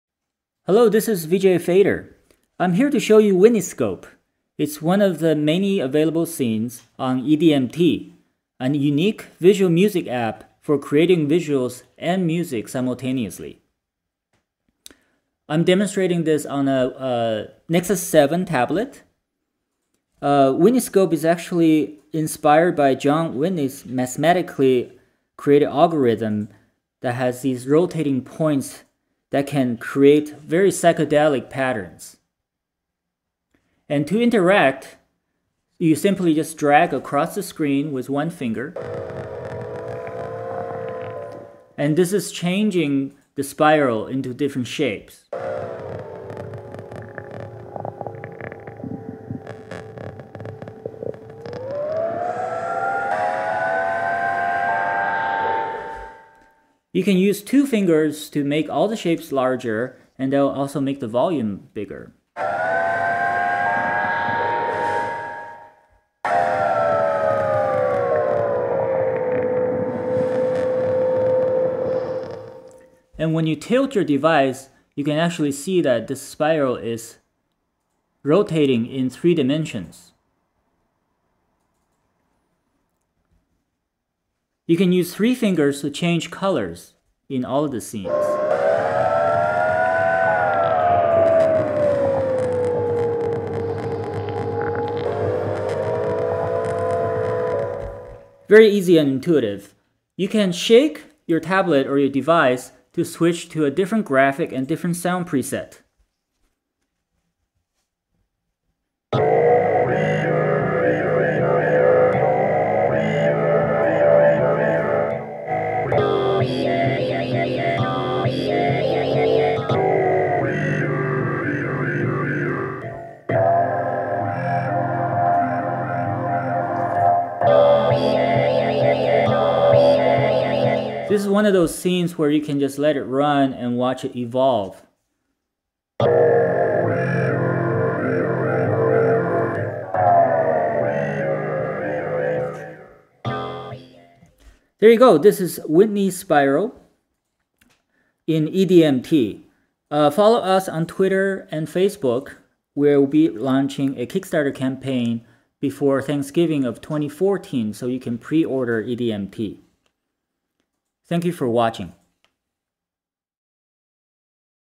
VJ Fader introduces EDMT - Whitney Scope
VJ Fader introduces Whitney Scope on EDMT App beta.
EDMT Kickstarter campaign runs from November 24th through December 24th 2014.